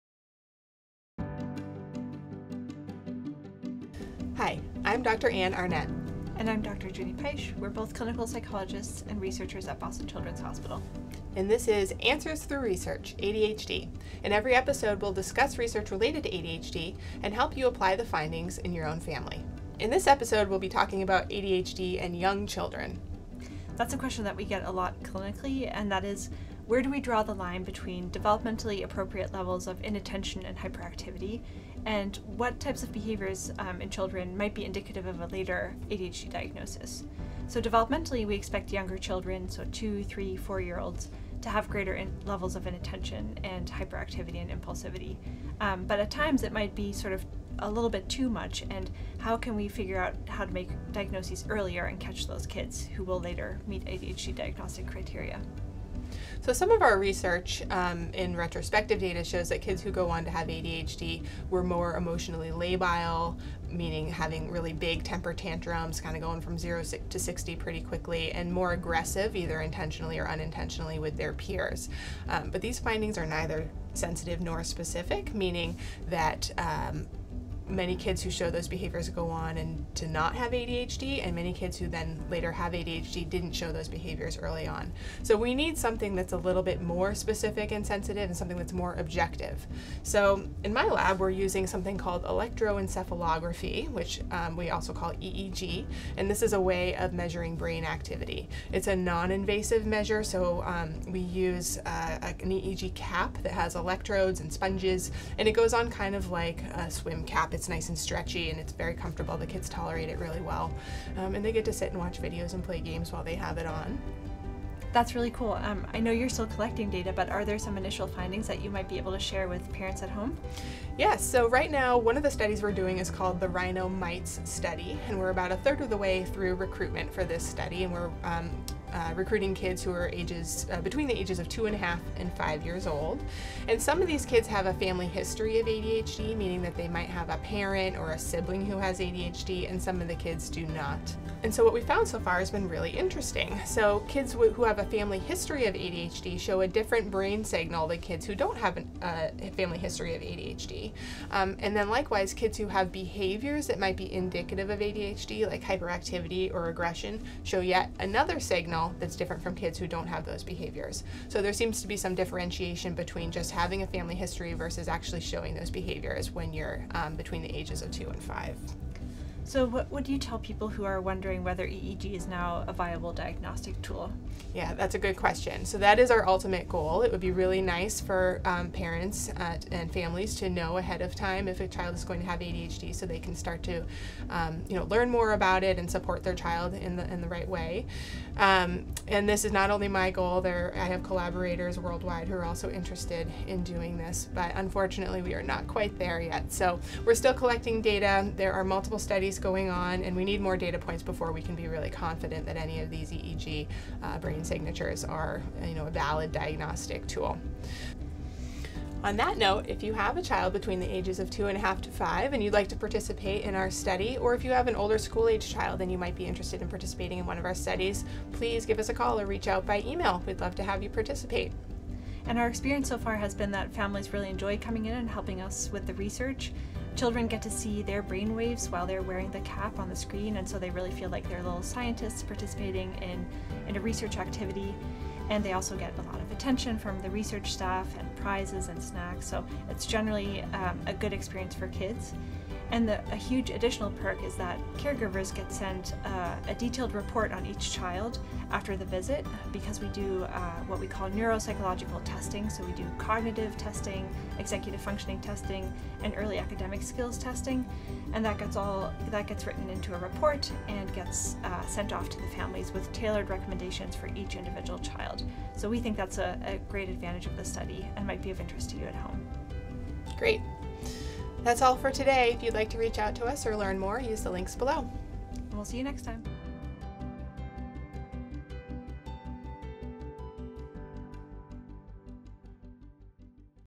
Answers through ADHD Research: ADHD in young children | Boston Children’s Hospital
Learn from Dr. Anne Arnett, PhD and Dr. Viriginia Peisch, PhD about ADHD in young children. To learn more about Attention Deficit and Hyperactivity Disorder (ADHD) please

▶Meet the Experts:

Anne Arnett, PhD
Clinical Psychologist and Scientist, Division of Developmental Medicine; Wade Family Investigator in Child Psychology

Dr. Virginia Peisch, PhD
Attending Psychologist, Division of Developmental Medicine